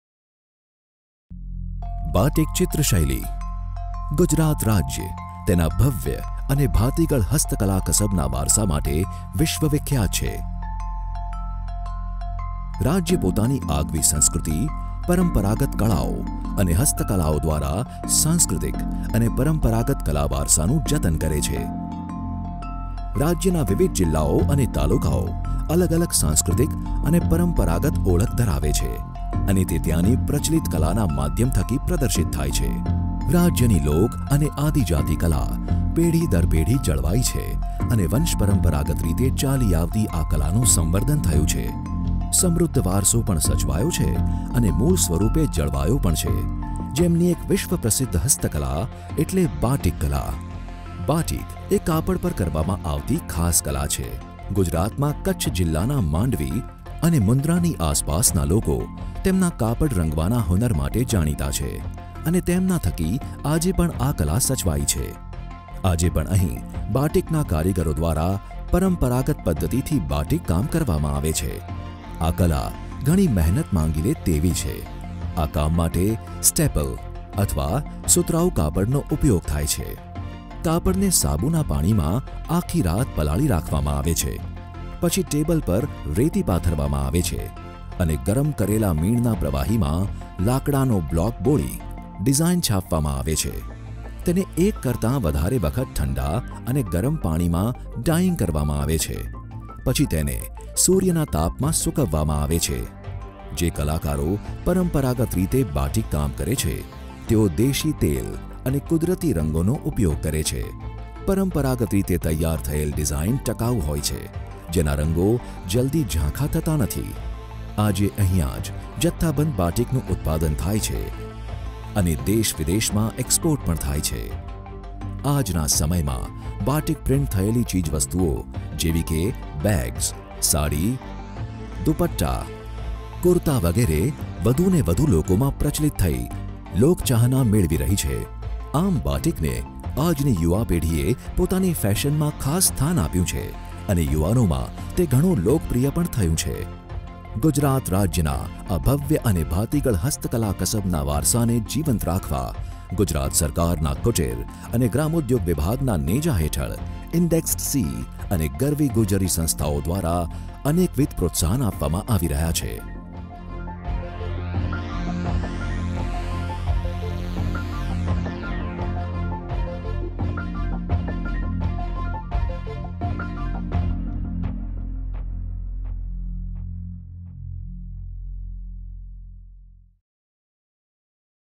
Batik Kala - બાટીક કળા
This video is about Gujarati Batik Art. I am not maker of this video. I have shared this video with the only intention to share the knowledge, ગમતા નો કરીએ ગુલાલ!